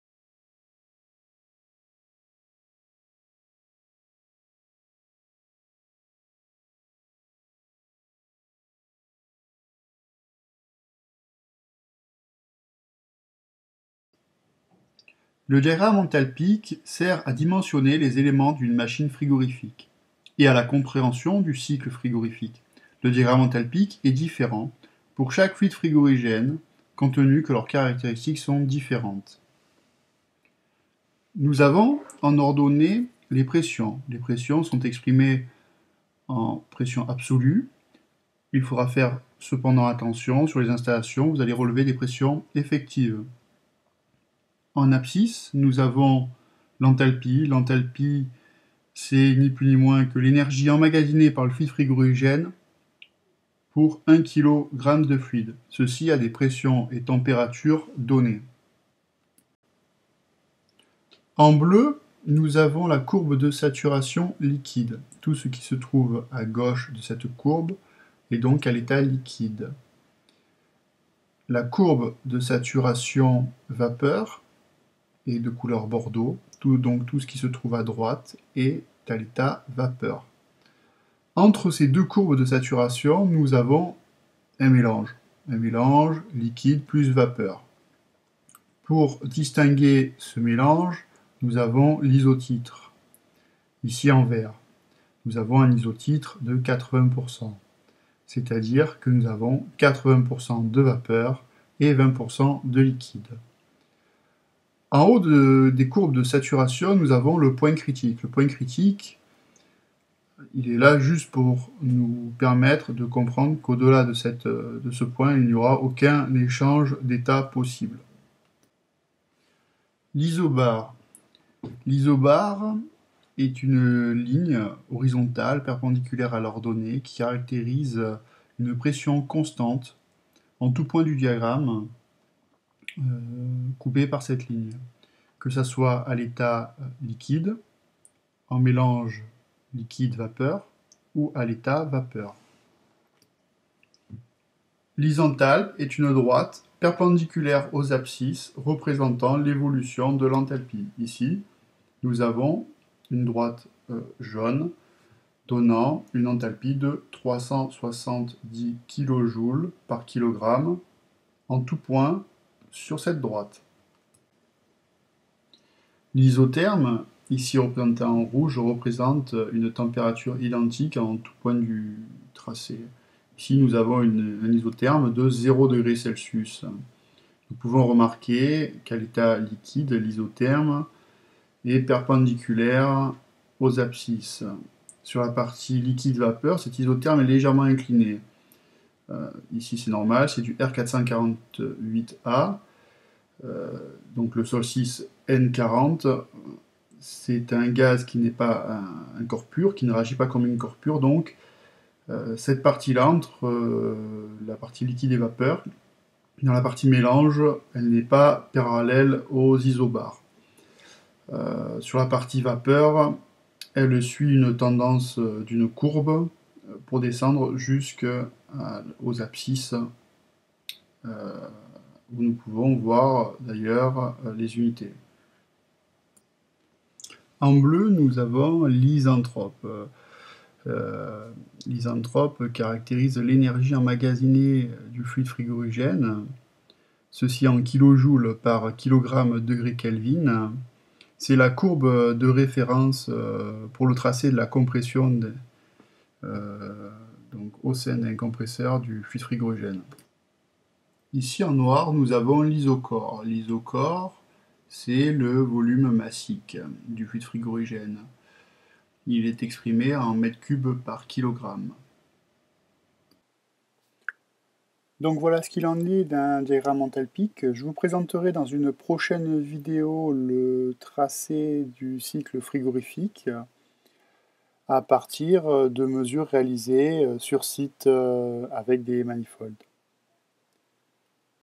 #2 présentation du diagramme ENTHALPIQUE
Présentation du diagramme ENTHALPIQUE aussi connu sous le nom de "MOLLIER".
- Utilité du diagramme
- Son utilisation
- Ses grandeurs physiques